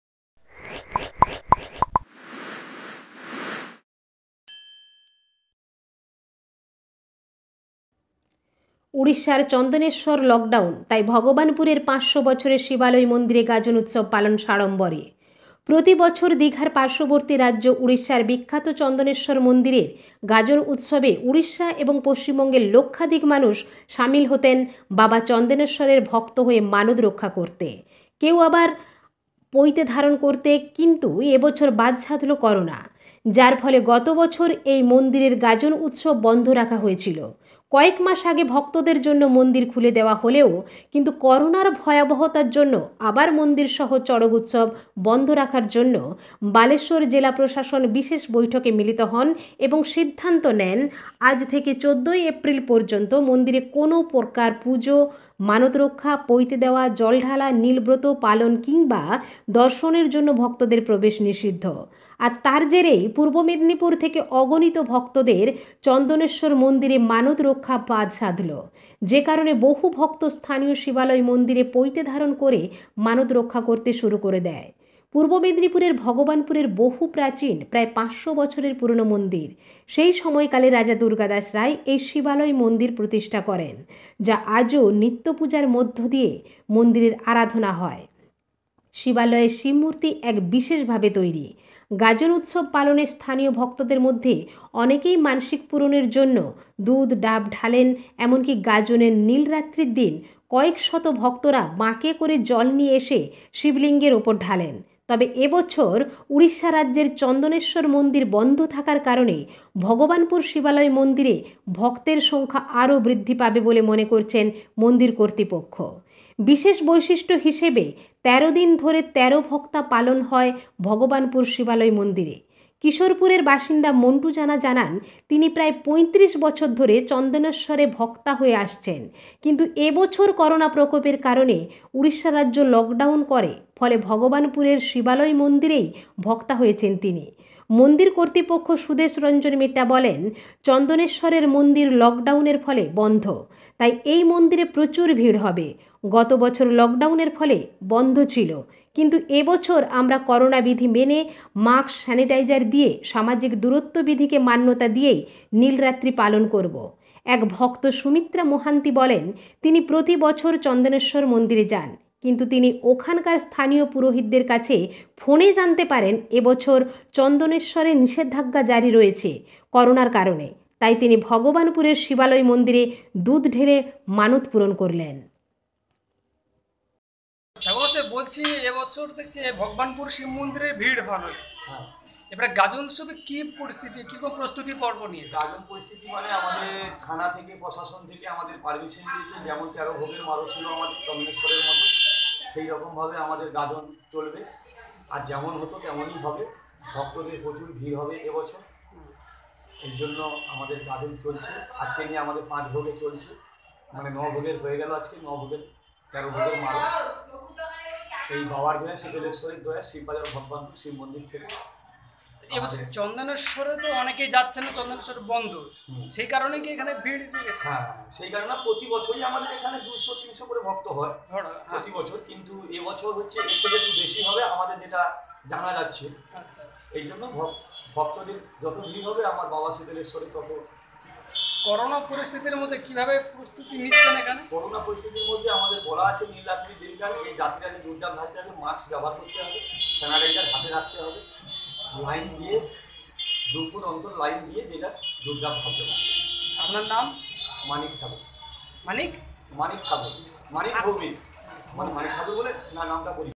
উড়িষ্যার চন্দনেশ্বর লকডাউন তাই ভগবানপুরের
উড়িষ্যার চন্দনেশ্বর লকডাউন তাই ভগবানপুরের পাঁচশো বছরের শিবালয় মন্দিরে গাজন উৎসব পালন সাড়ম্বরে
প্রতি বছর দীঘার পার্শ্ববর্তী রাজ্য ঊড়িষ্যার বিখ্যাত চন্দনেশ্বর মন্দিরের  গাজন উৎসবে ঊড়িষ্যা এবং পশ্চিম বঙ্গের লক্ষাধিক মানুষ সামিল হতেন বাবা চন্দনেশ্বরের ভক্ত হয়ে মানত রক্ষা করতে , কেউ কেউ আবার পৈতে ধারণ করতে কিন্তু এবছর  বাধ সাধল করোনা। যাঁর ফলে গচ বছর এই মন্দিরের গাজন উৎসব বন্ধ রাখা হয়েছিল। কয়েক মাস আগে ভক্তদের জন্য মন্দির খুলে দেওয়া হলেও কিন্তু করোনার ভয়াবহতার জন্য আবার মন্দির সহ চড়ক উৎসব বন্ধ রাখার জন্য বালেশ্বর জেলা প্রশাসন বিশেষ বৈঠকে মিলিত হয় এবং সিদ্ধান্ত নেয় আজ থেকে১৪ ই এপ্রিল পর্যন্ত মন্দিরে কোনো প্রকার পূজা ,মানত রক্ষা,পৈতে দেওয়া,জলঢালা ,নীল ব্রত পালন কিংবা দর্শনের জন্য ভক্তদের প্রবেশ নিষিদ্ধ ।
আর তাঁর জেরেই পূর্ব মেদিনীপুর থেকে অগণিত ভক্তদের চন্দনেশ্বর মন্দিরে মানত রক্ষা বাধ সাধল। যে কারনে বহু ভক্ত স্থানীয় শিবালয় মন্দিরে পৈতে ধারণ করে মানত রক্ষা করতে শুরু করে দেয়।
পূর্ব মেদিনীপুরের ভগবানপুরের বহু প্রাচীন প্রায় পাঁচশো বছরের পুরানো মন্দির, সেই সময়কালে রাজা দুর্গাদাস রায় এই শিবালয় মন্দিরের প্রতিষ্ঠা করেন। যা আজও নিত্য পূজার মধ্য দিয়ে মন্দিরের আরাধনা হয়। শিবালয়ের শিব মূর্তি এক বিশেষ ভাবে তৈরি। গাজন উৎসব পালনে স্থানীয় ভক্তদের মধ্যে অনেকেই মানসিক পুরনের জন্য দুধ, ডাব, ঢালেন, এমনকি গাজনের নীল রাত্রির দিন কয়েকশত ভক্তরা বাঁকে করে জল নিয়ে এসে শিবলিঙ্গের ওপর ঢালেন, তবে এবছর উড়িষ্যা রাজ্যের চন্দনেশ্বর মন্দির বন্ধ থাকার কারনে ভগবানপুর শিবালয় মন্দিরে ভক্তের সংখ্যা আরও বৃদ্ধি পাবে বলে মনে করছেন মন্দির কর্তৃপক্ষ। বিশেষ বৈশিষ্ট্য হিসেবে তেরোদিন ধরে তেরো ভক্তা পালন হয় ভগবানপুর শিবালয় মন্দিরে। কিশোরপুরের বাসিন্দা মন্টু জানা জানান তিনি প্রায় পঁয়ত্রিশ বছর ধরে চন্দনেশ্বরে ভক্তা হয়ে আসছেন, কিন্তু এবছর করোনা প্রকোপের কারনে উড়িষ্যা রাজ্য লকডাউন করে ফলে ভগবানপুরের শিবালয় মন্দিরেই ভক্তা হয়েছেন তিনি।
মন্দির কর্তৃপক্ষ সুদেশ রঞ্জন মেট্যা বলেন চন্দনেশ্বরের মন্দির লকডাউনের ফলে বন্ধ, তাই এই মন্দিরে প্রচুর ভিড় হবে, গতবছর লকডাউনের ফলে বন্ধ ছিল কিন্তু এ বছর আমরা করোনা বিধি মেনে মাক্স, স্যানিটাইজার দিয়ে সামাজিক দূরত্ব বিধিকে মান্যতা দিয়েই নীলরাত্রি পালন করব।
এক ভক্ত সুমিত্রা মোহান্তি বলেন তিনি প্রতিবছর চন্দনেশ্বর মন্দিরে যান, কিন্ত তিনি ওখানকার স্থানীয় পুরোহিতের কাছে ফোনে জানতে পারেন এ বছর চন্দনেশ্বরে নিষেধাজ্ঞা জারি রয়েছে করোনার কারনে ,তাই তিনি ভগবানপুরের শিবালয় মন্দিরে দুধ ঢেলে মানত পুরন করলেন।